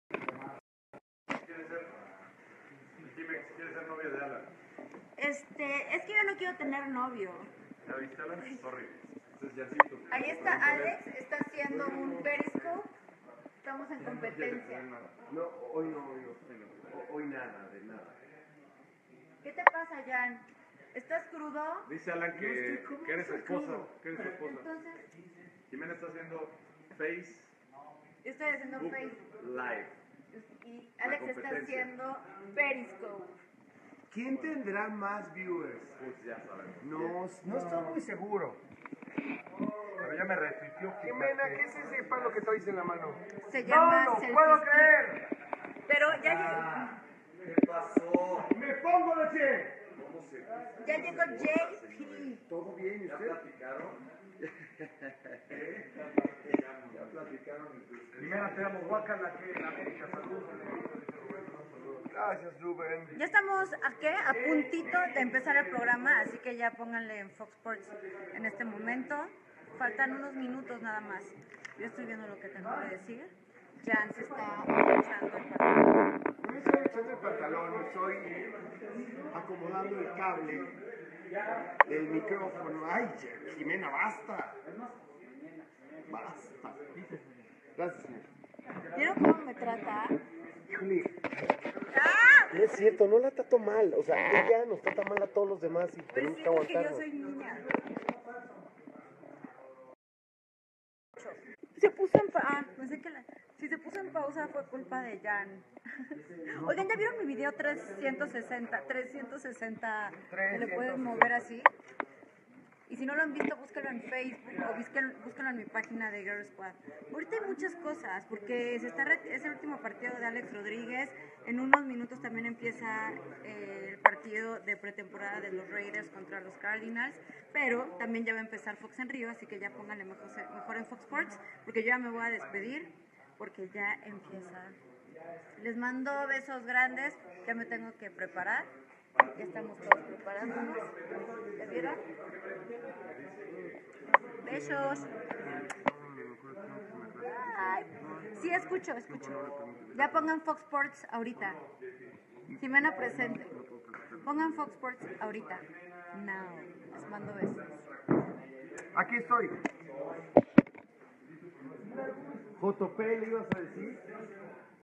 Jimena Sánchez - Transmisión En Directo Facebook Live 12 De Agosto 2016
Video de Jimena Sanchez que transmitió en vivo en su fanpage de Facebook el día 12 De Agosto De 2016